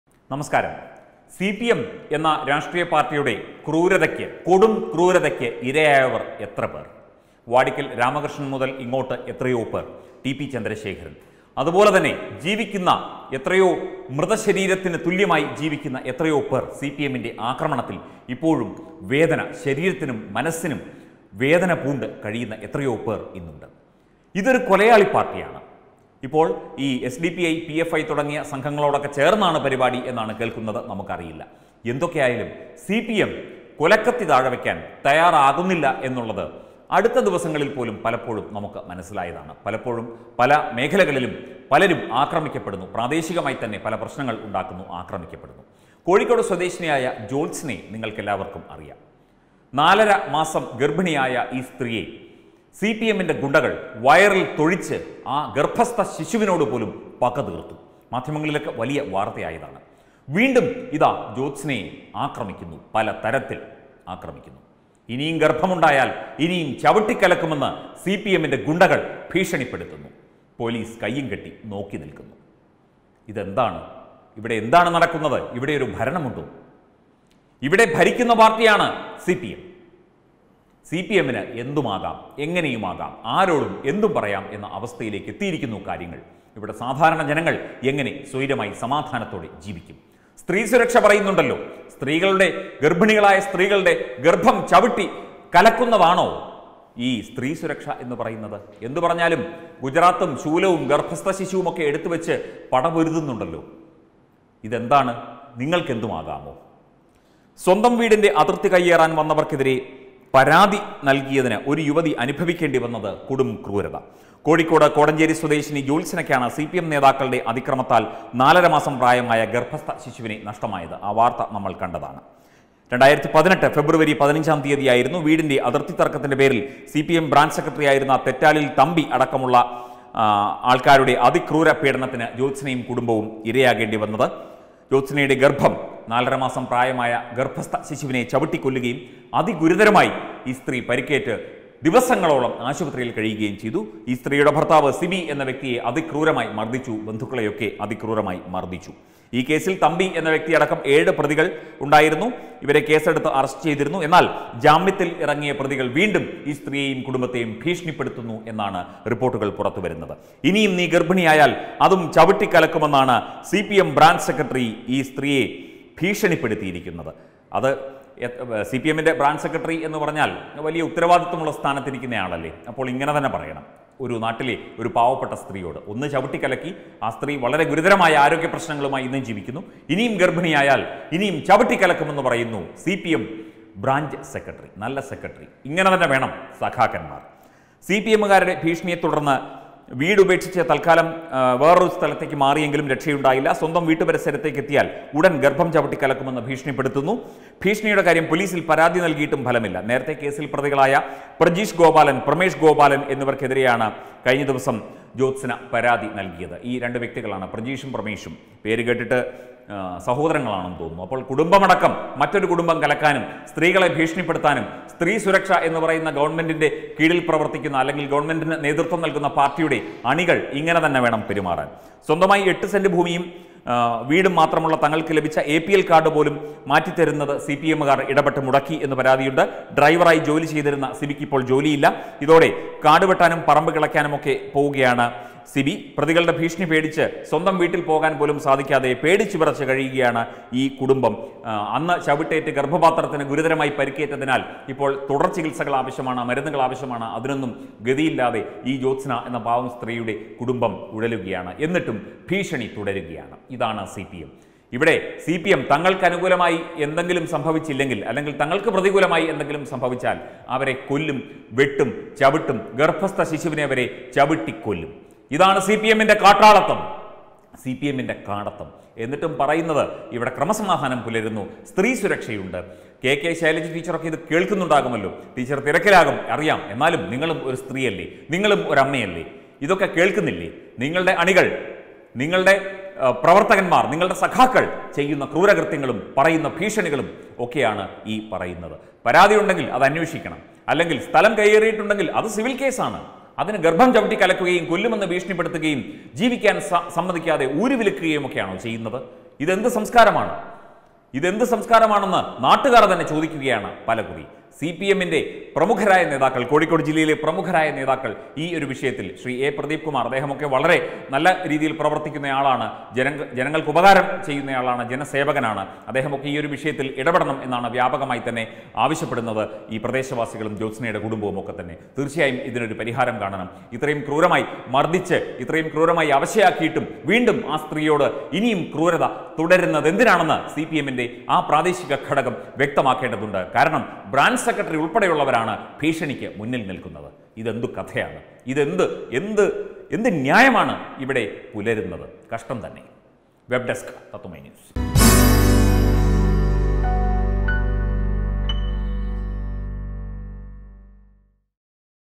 ഗർഭമുണ്ടായാൽ ഇനിയും ചവിട്ടിക്കലക്കും...സി പി എം ഗുണ്ടകളുടെ പരാക്രമം വീണ്ടും...നല്ല സ്ത്രീ സുരക്ഷ...
കോഴിക്കോട് സ്വദേശിനി ജ്യോത്സ്‌നയോട് വീണ്ടും പക വീട്ടുന്നു,സി പി എം ഗുണ്ടാ സംഘം.പോലീസ് കയ്യും കെട്ടി നോക്കി നിൽക്കുന്നു...